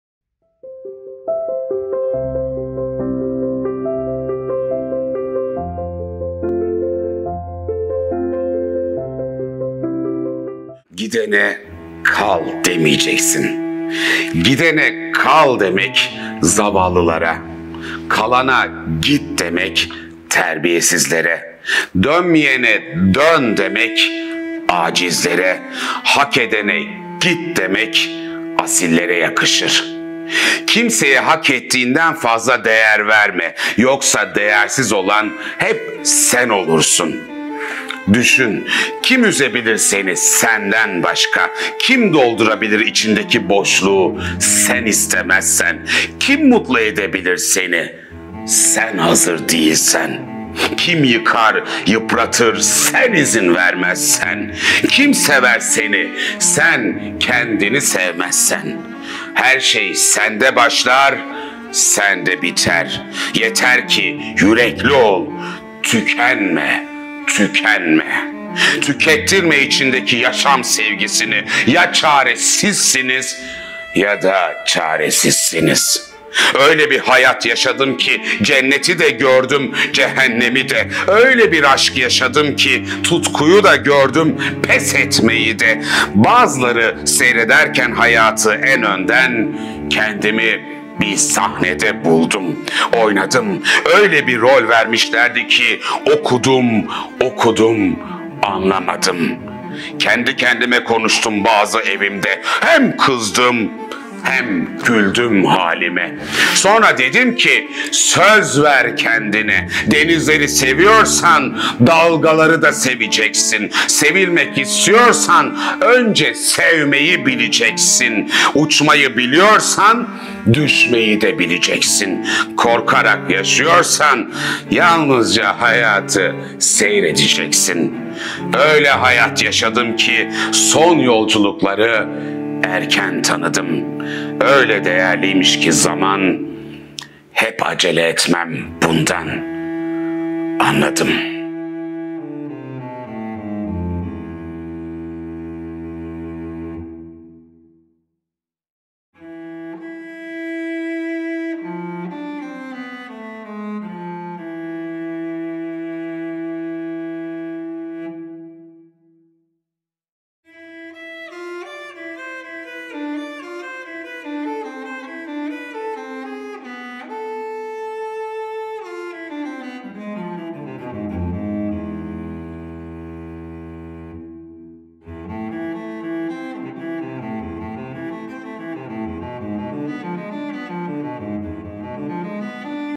Gidene Kal Demeyeceksin-Friedrich Nietzsche //Şiir Arası
Tiyatro Sanatçısı ve Yazar Utku Erişik, Nietzsche'nin "Gidene Kal Demeyeceksin" şiirini seslendiriyor.

Gidene kal demeyeceksin. ..

Gidene kal demek zavallılara,
Kalana git demek terbiyesizlere,
Dönmeyene dön demek acizlere,
Hak edene git demek asillere yakışır.

Kimseye hak etmediğinden fazla değer verme,
yoksa değersiz olan hepsen olursun...

Düşün...

Kim üzebilir seni senden başka?
Kim doldurabilir içindeki boşluğu sen istemezsen?
Kim mutlu edebilir seni, sen hazır değilsen?
Kim yıkar, yıpratır sen izin vermezsen?
Kim sever seni, sen kendini sevmezsen?
Her şey sende başlar, sende biter...

Yeter ki yürekli ol, tükenme, tüketme, tükettirme içindeki yaşama sevgisini...

Ya çare sizsiniz yada çaresizsiniz. ..
Öyle bir hayat yaşadım ki cenneti de gördüm cehennemi de.
Öyle bir aşk yaşadım ki tutkuyu da gördüm pes etmeyi de.
Bazıları seyrederken hayatı en önden, kendimi bir sahnede buldum,
Oynadım.

Öyle bir rol vermişlerdi ki okudum okudum anlamadım.
Kendi kendime konuştum bazen evimde,
hem kızdım hem güldüm halime.
Sonra dedim ki söz ver kendine
Denizleri seviyorsan dalgaları da seveceksin,
Sevilmek istiyorsan önce sevmeyi bileceksin,
Uçmayı biliyorsan düşmeyi de bileceksin,
Korkarak yaşıyorsan yalnızca hayatı seyredeceksin.
Öyle hayat yaşadım ki son yolculukları erken tanıdım.
Öyle değerliymiş ki zaman hep acele etmem bundan anladım...